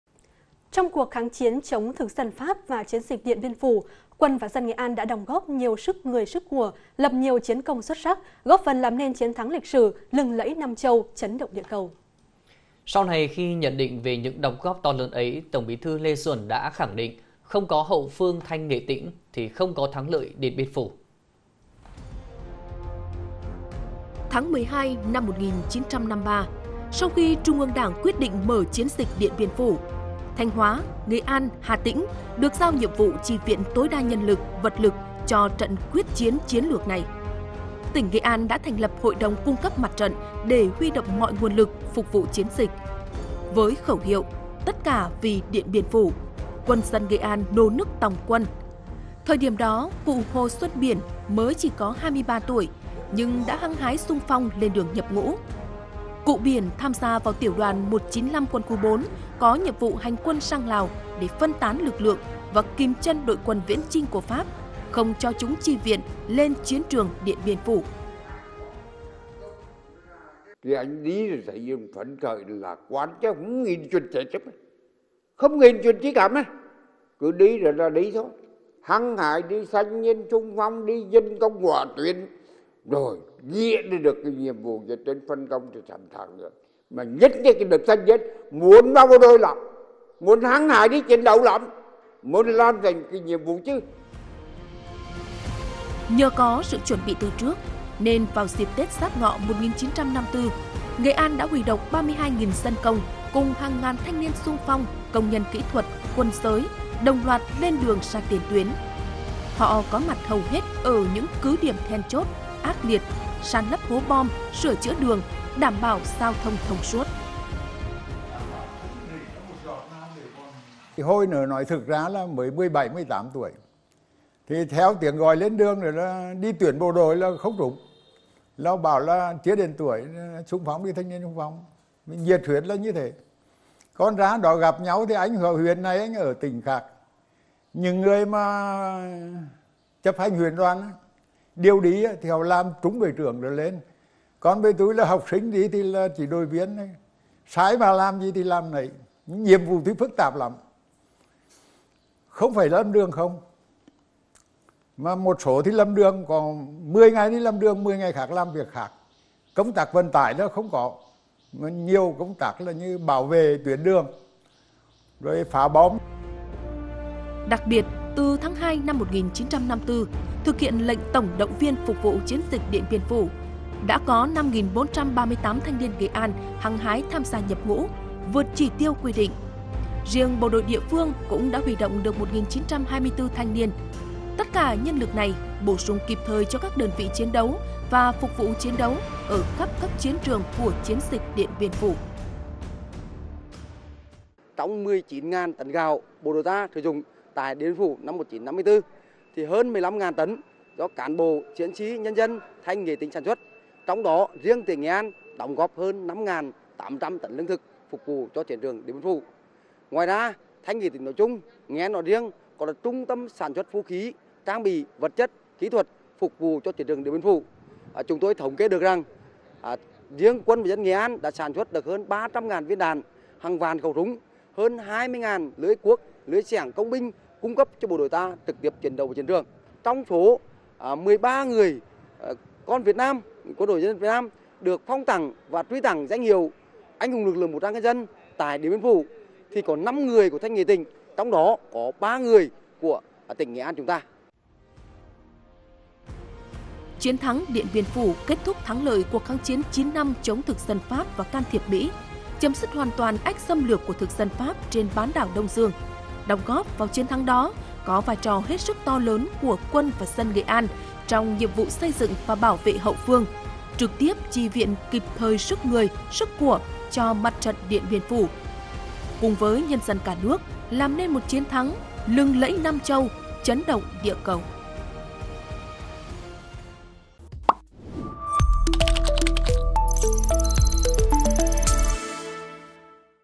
Đóng góp của quân và dân Nghệ An trong chiến dịch Điện Biên Phủ
Trong cuộc kháng chiến chống thực dân Pháp và chiến dịch Điện Biên Phủ, quân và dân Nghệ An đã đóng góp nhiều sức người sức của, lập nhiều chiến công xuất sắc, góp phần làm nên chiến thắng lịch sử “Lừng lẫy năm châu, chấn động địa cầu”. Sau này khi nhận định về những đóng góp to lớn ấy, Tổng bí thư Lê Duẩn đã khẳng định: “Không có hậu phương Thanh-Nghệ-Tĩnh thì không có thắng lợi Điện Biên Phủ”.